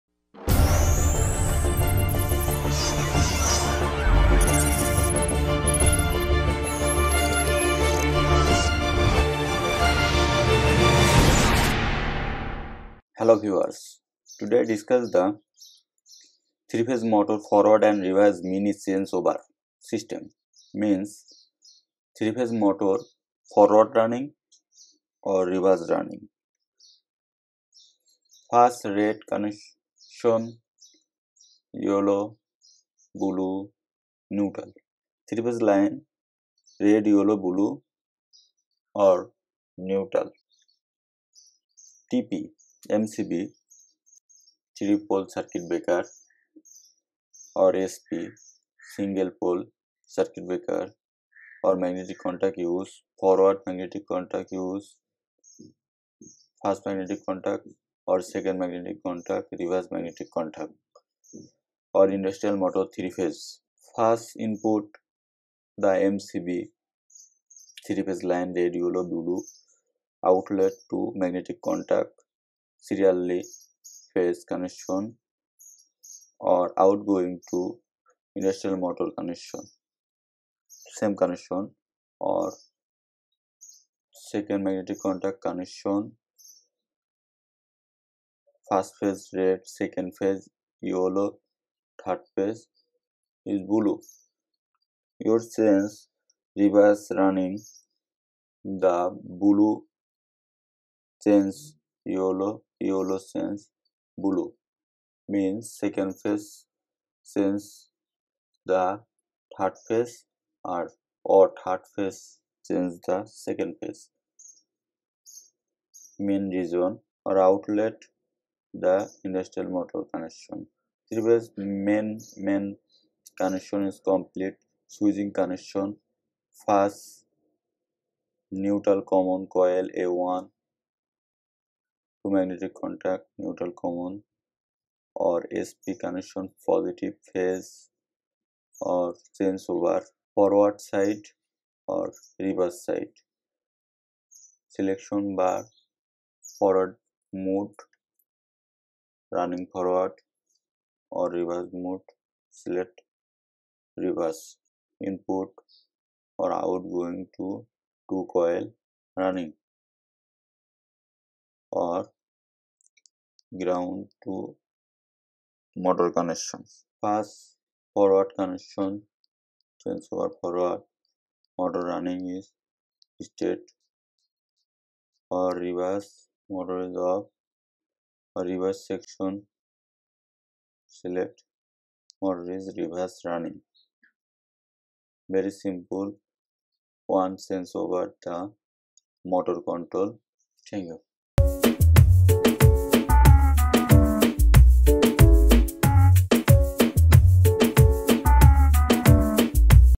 3 Phase Motor Forward Reverse Mini Changeover
This video shows 3 Phase Motor Forward Reverse Mini Changeover. forward reverse motor control is used in a system where forward and backward or upward and downward movement in the operation is needed. Forward and Reverse Operation of the motor can be obtained by interchanging any two of its three terminals.
Did you video miss it?